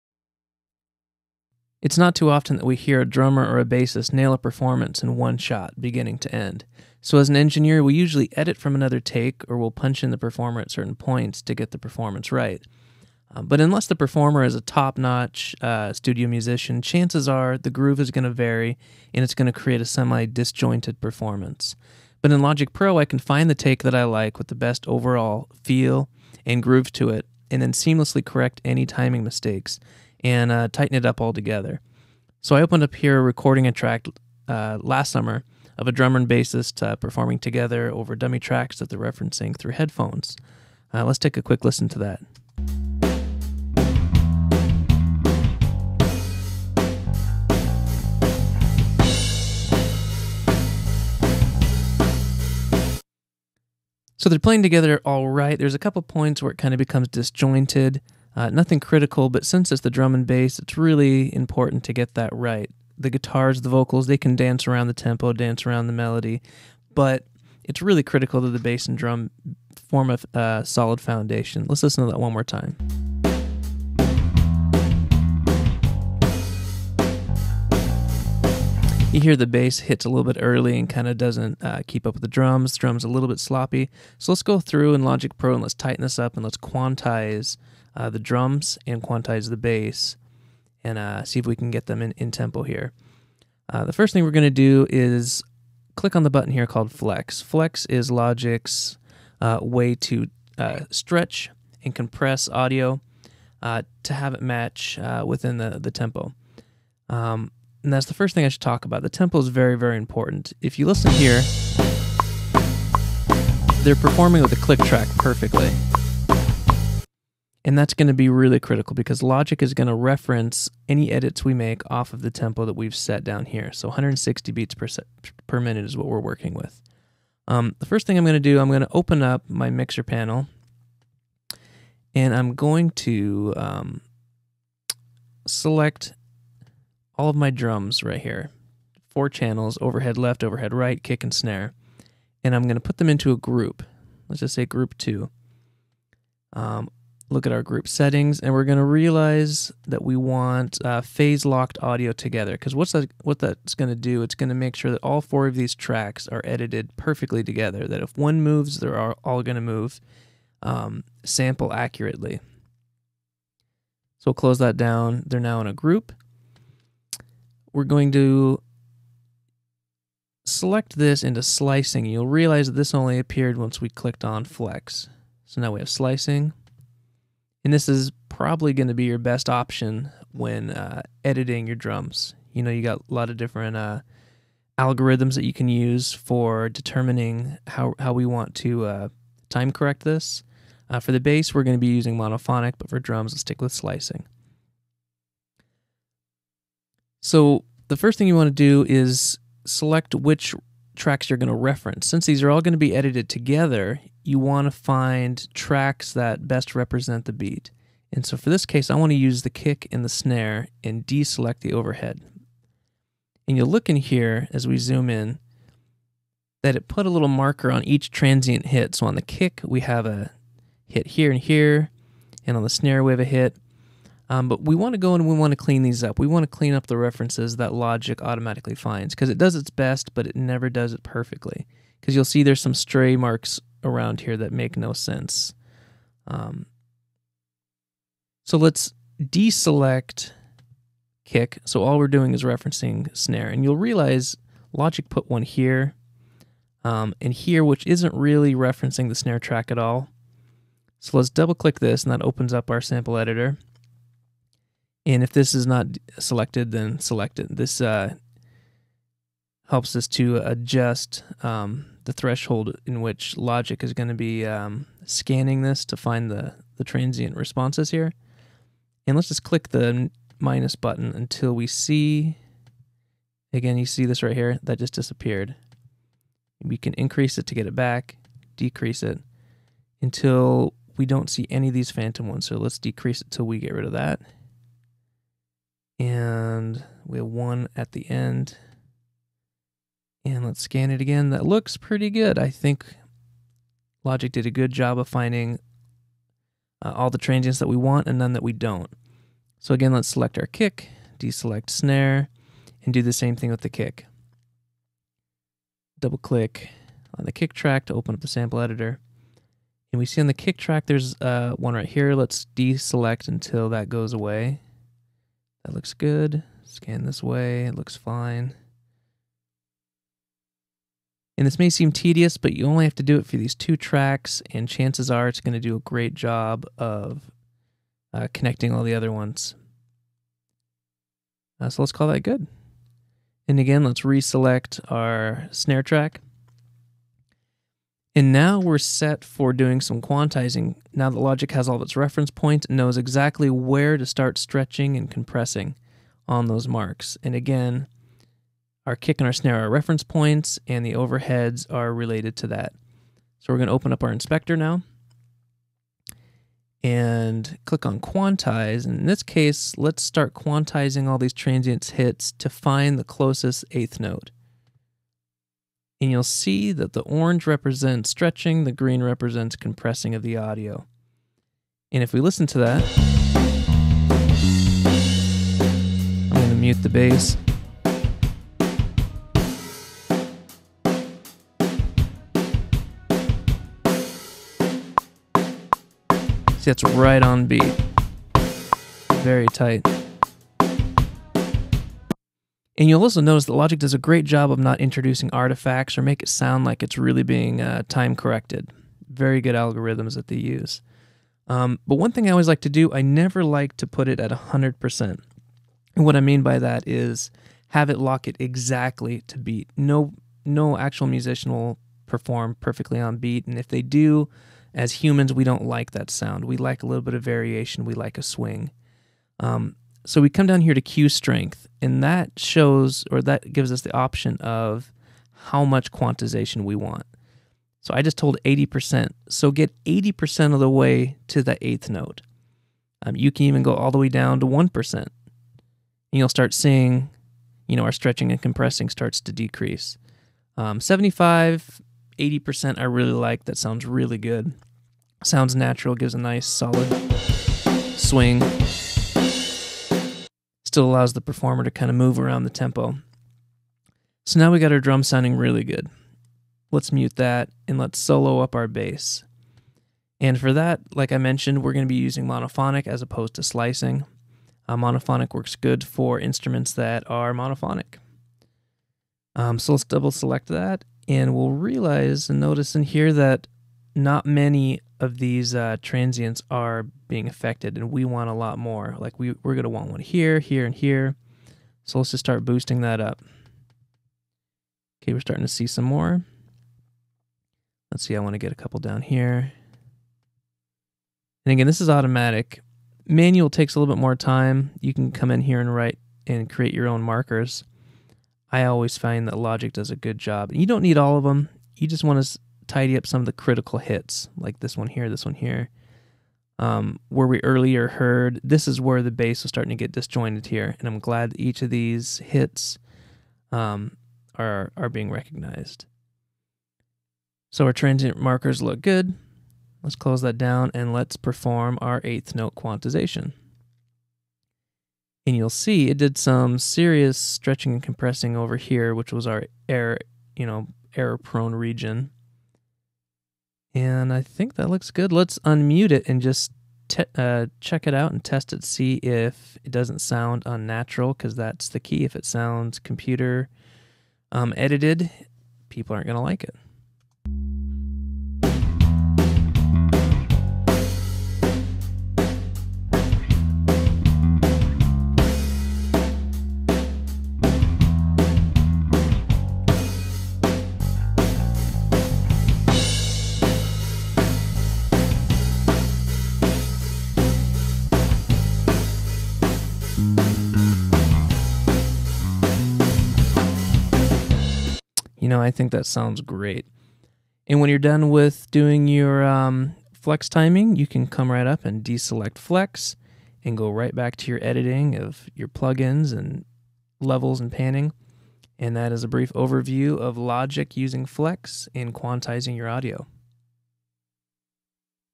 Quantizing Audio in Logic 9
A brief overview of quantizing drum and bass using Flex in Logic Pro 9.